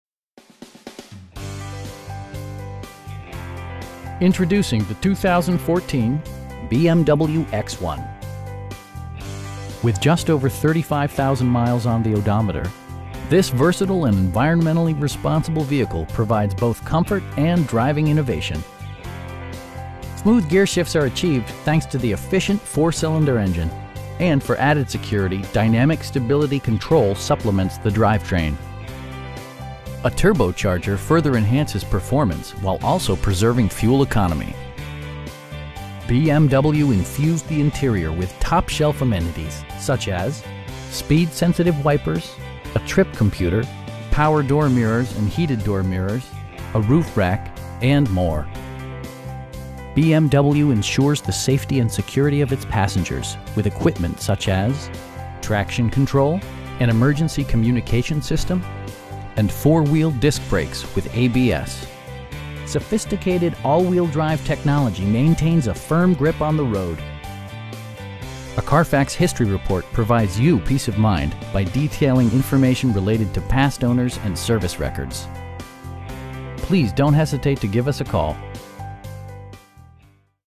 2014 BMW X1 xDrive28i in Dedham, MA 02026
Introducing the 2014 BMW X1. With just over 35,000 miles on the odometer, this versatile and environmentally responsible vehicle provides both comfort and driving innovation! Smooth gearshifts are achieved thanks to the efficient 4 cylinder engine, and for added security, dynamic Stability Control supplements the drivetrain. A turbocharger further enhances performance, while also preserving fuel economy. BMW infused the interior with top shelf amenities, such as: speed sensitive wipers, a trip computer, tilt steering wheel, power door mirrors and heated door mirrors, power windows, a roof rack, and more. BMW ensures the safety and security of its passengers with equipment such as: dual front impact airbags with occupant sensing airbag, front side impact airbags, traction control, brake assist, anti-whiplash front head restraints, a security system, an emergency communication system, and 4 wheel disc brakes with ABS. Sophisticated all wheel drive technology maintains a firm grip on the road. A Carfax history report provides you peace of mind by detailing information related to past owners and service records. We know that you have high expectations, and we enjoy the challenge of meeting and exceeding them! Please don't hesitate to give us a call.